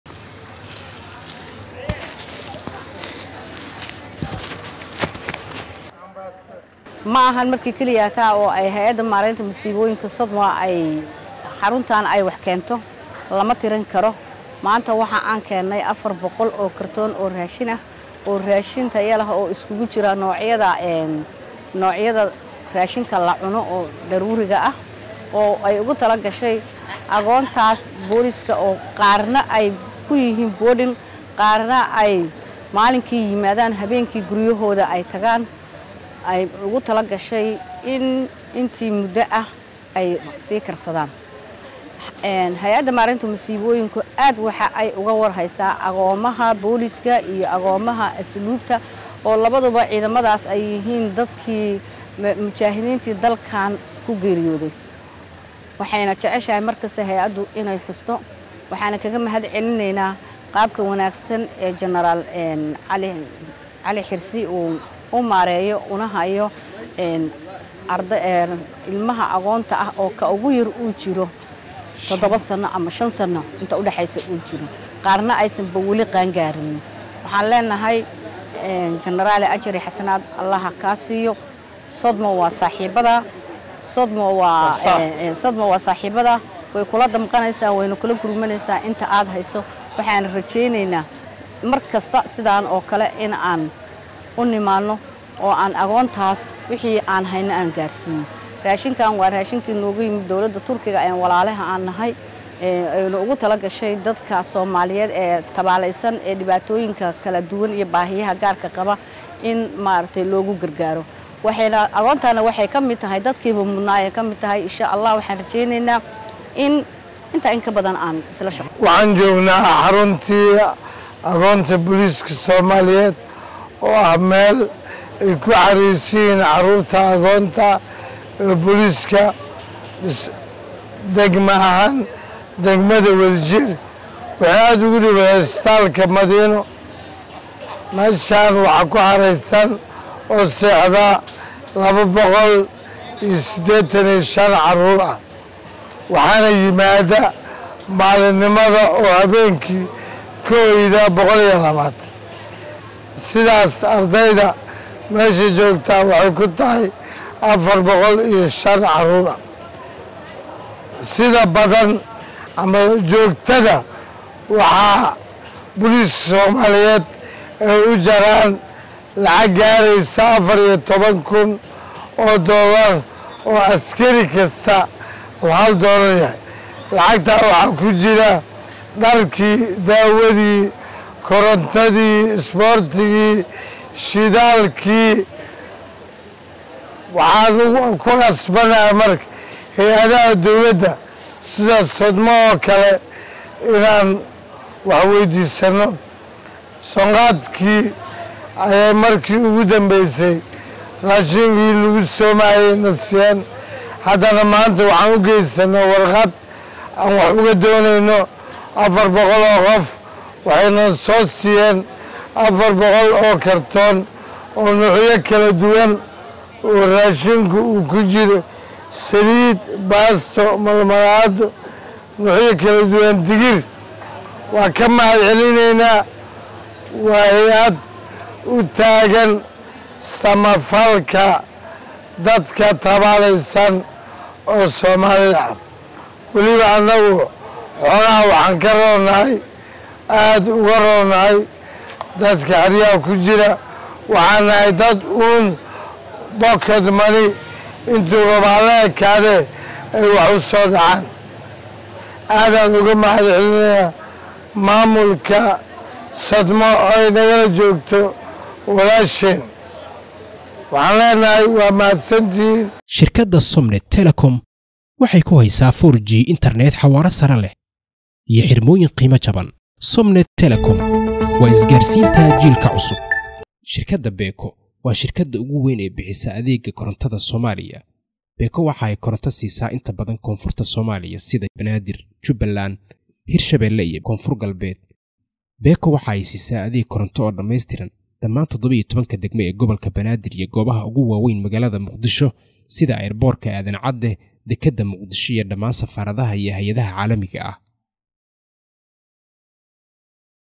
Hay'adda Maareynta Musiibooyinka Qaranka ee SoDMA oo deeq gargaar ah gaarsiisay Xarunta Barbaarinta
Hay'adda Maareynta Musiibooyinka Qaranka ee SoDMA oo deeq gargaar ah gaarsiisay Xarunta Barbaarinta Carruurta agoonta Booliiska Soomaaliyeed.